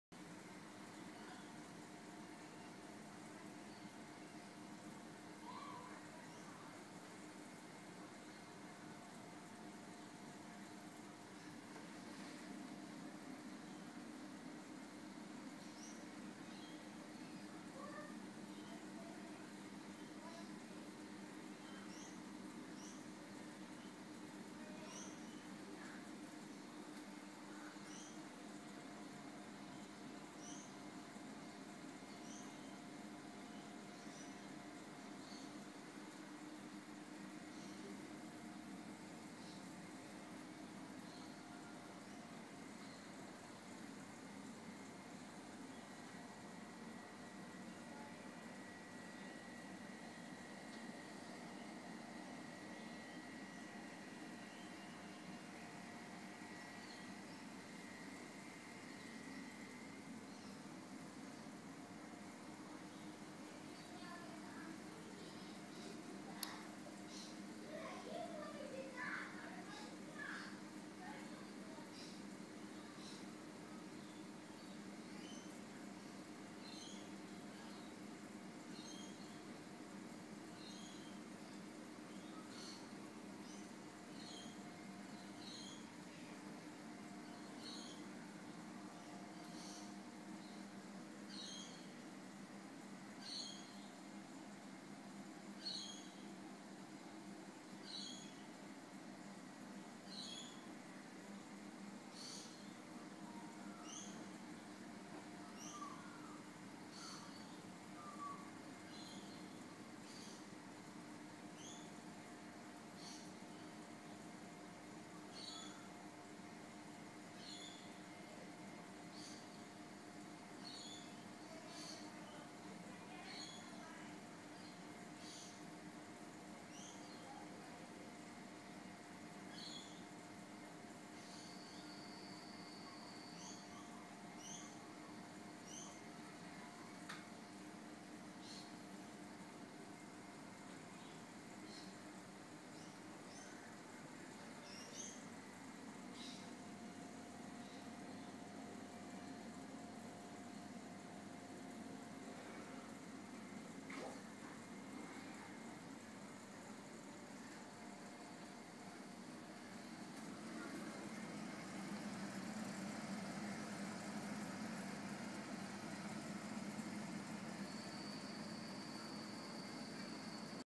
Big shark eppaulette in home predator tank!
New speckled eppaulette shark just added to my tank!